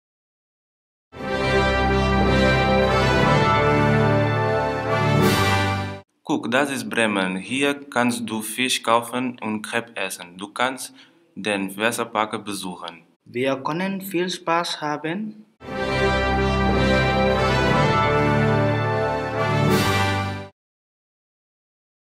Schönes Bremen
Ein Vorkurs des TBZ Bremen produzierte Trickfilme mit "Bremer Szenen"